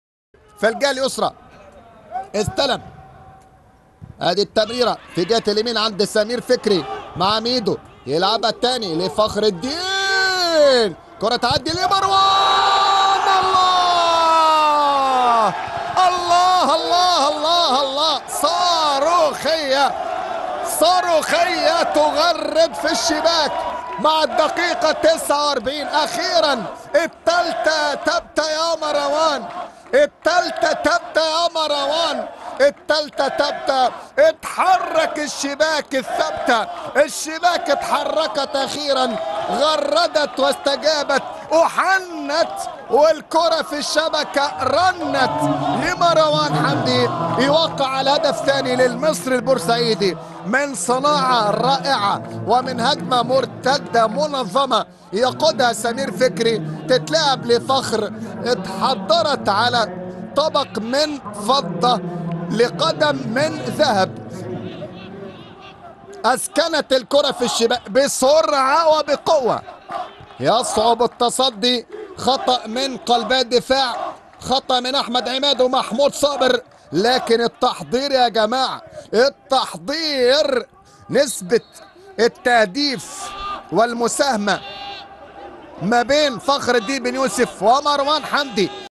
بتسدية صاروخية مروان حمدي يحرز الهدف التاني للمصري ( الجولة الثالثة)دوري النيل 2023-2024
Al Masry VS El-Dakhleya